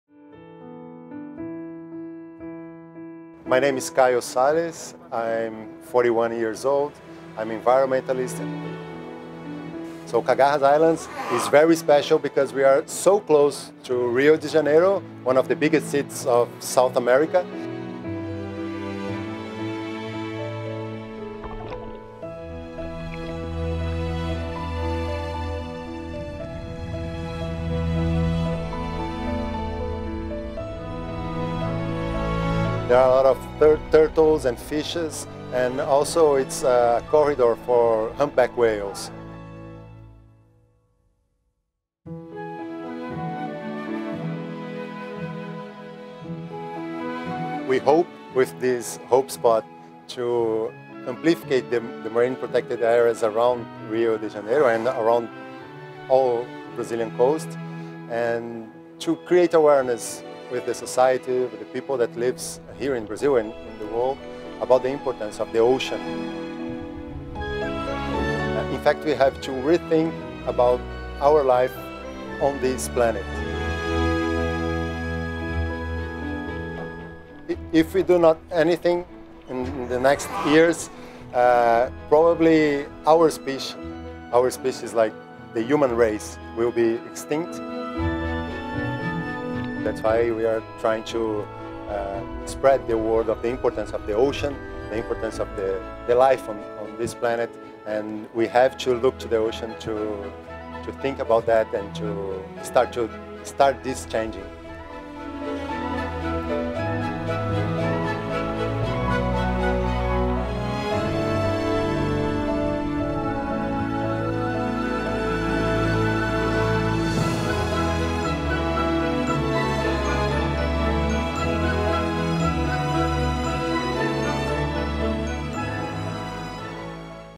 Saving Brazil's marine life
Brazil is the most biologically diverse country in the world, and while the COVID-19 outbreak continues to hit the country hard, Rio de Janeiro has something to commemorate.